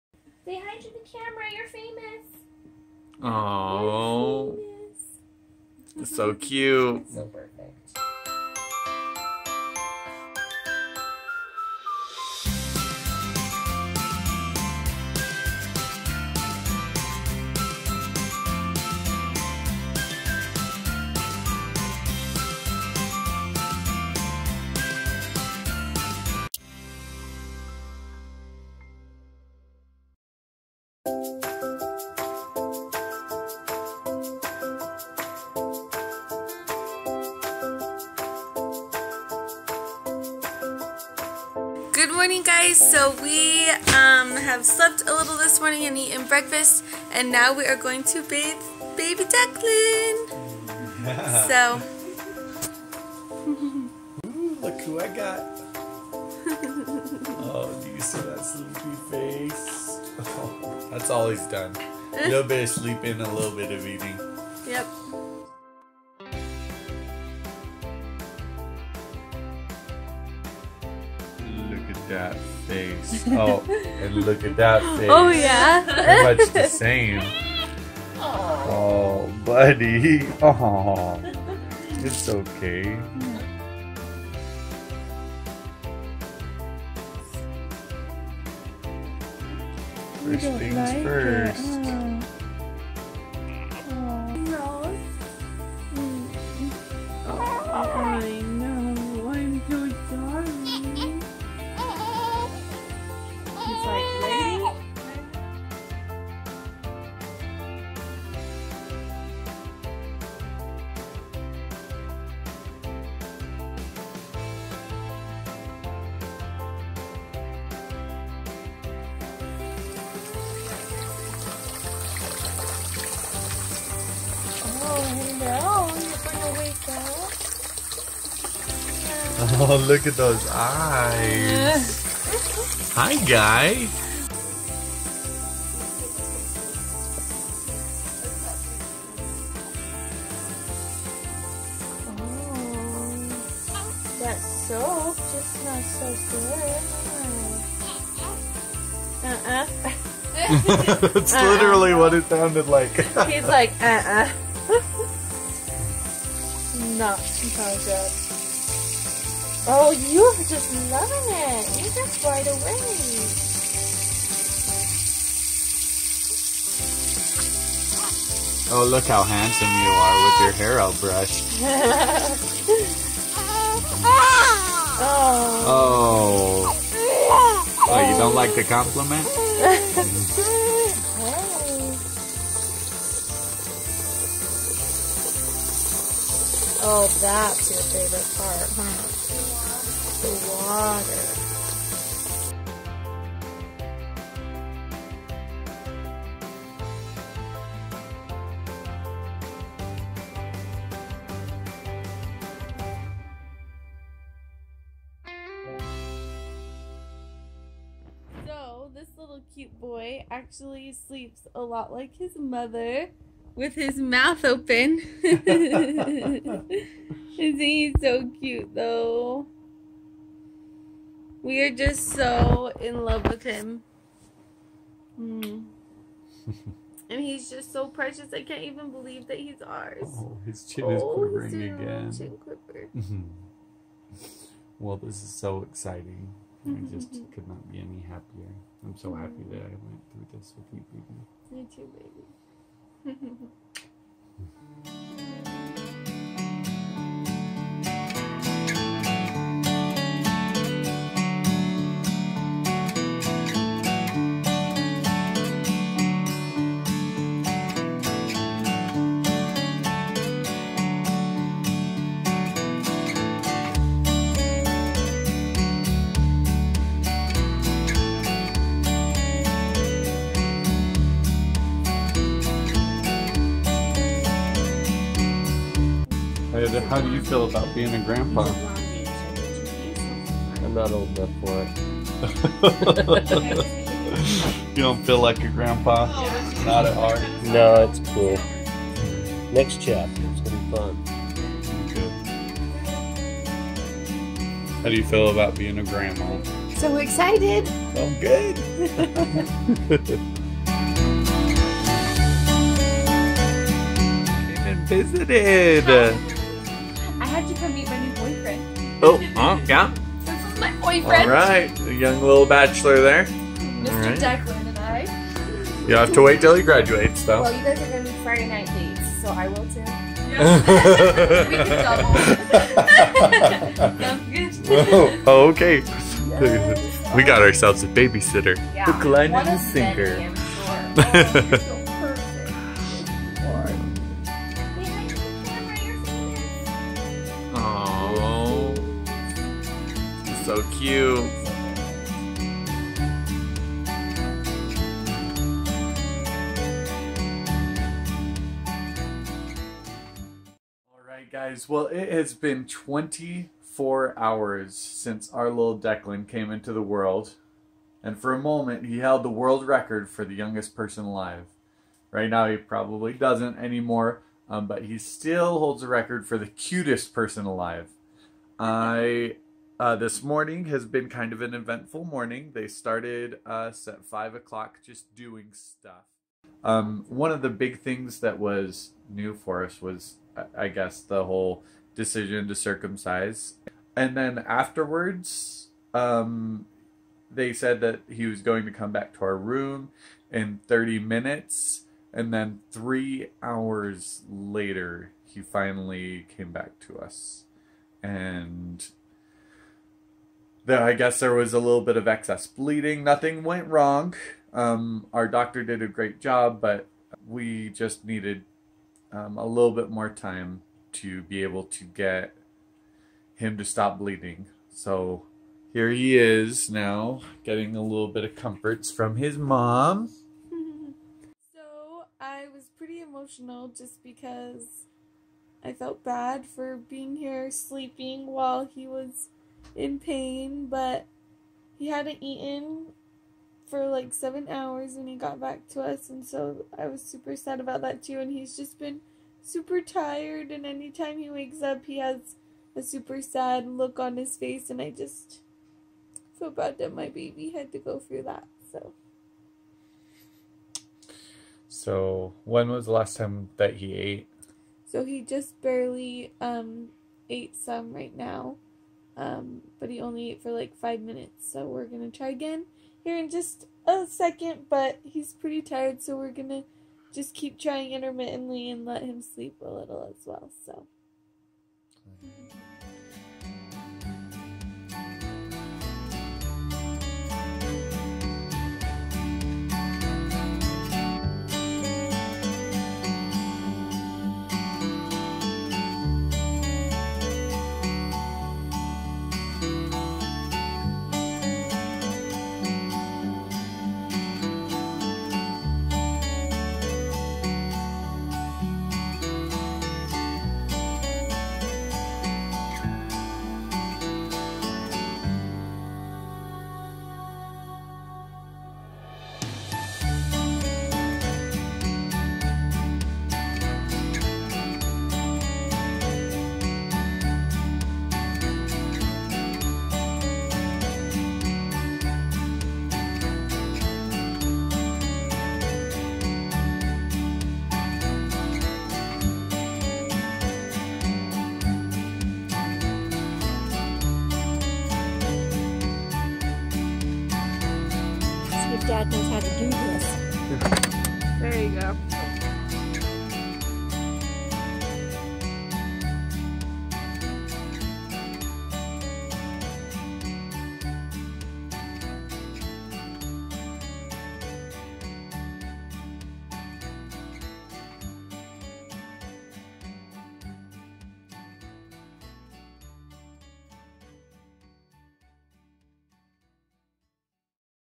The Beginnings of the Life of Declan
Take a look at our stay in the hospital and a few of the loved ones that came to see Declan! He was so new and shiny!

(6) Joel
(7) University of Utah: